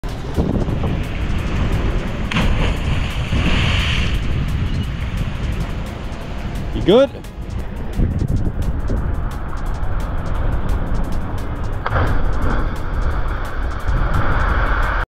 HE SENT IT OFF THIS 12 METRE CLIFF.
This jump is near Watson's Bay in Sydney and is higher than it looks.